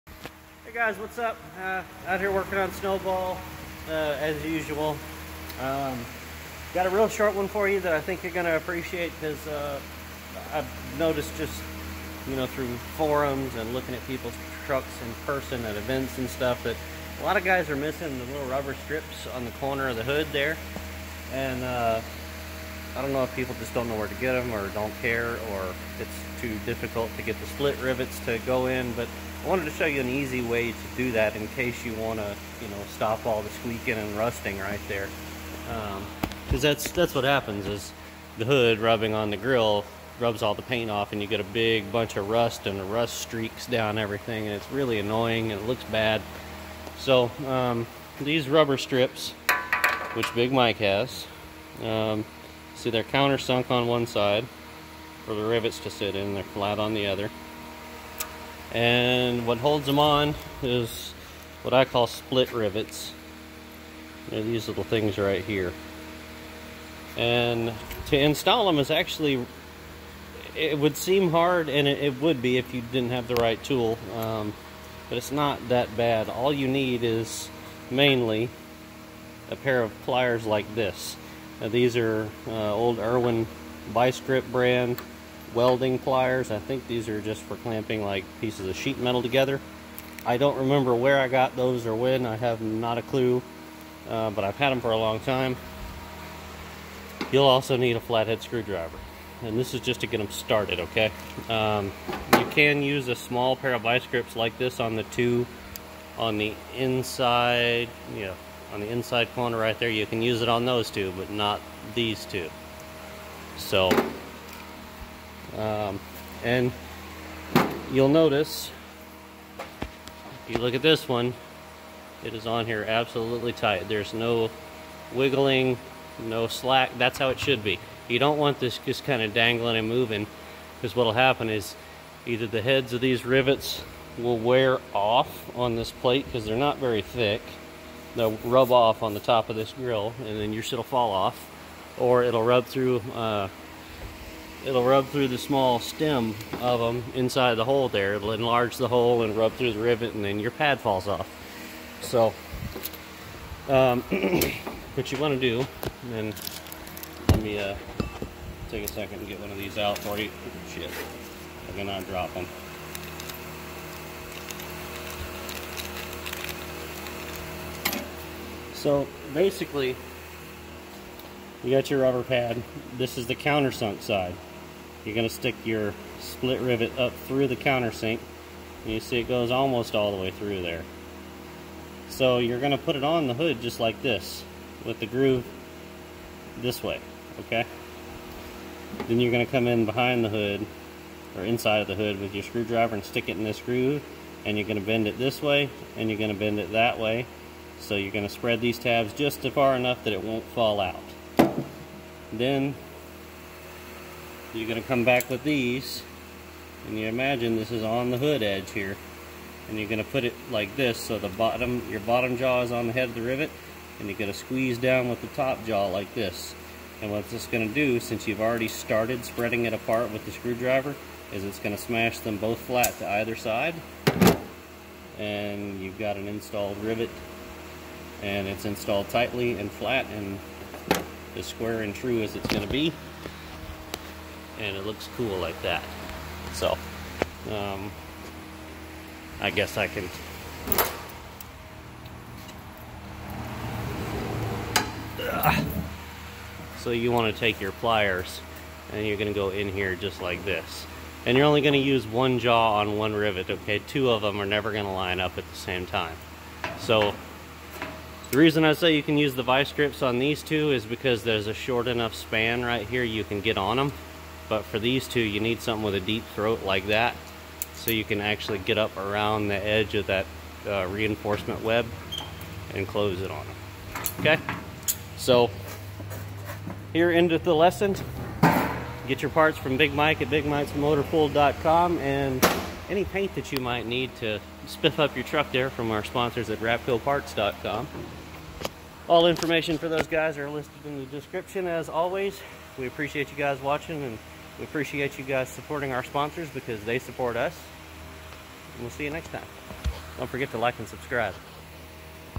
Installing M35 hood to grille anti-squeak strips and rivets.
Can't possibly be more descriptive than the title...

Label your donation as " donation" or " support" so we don't get our donations confused with our used parts customers. We want to thank you!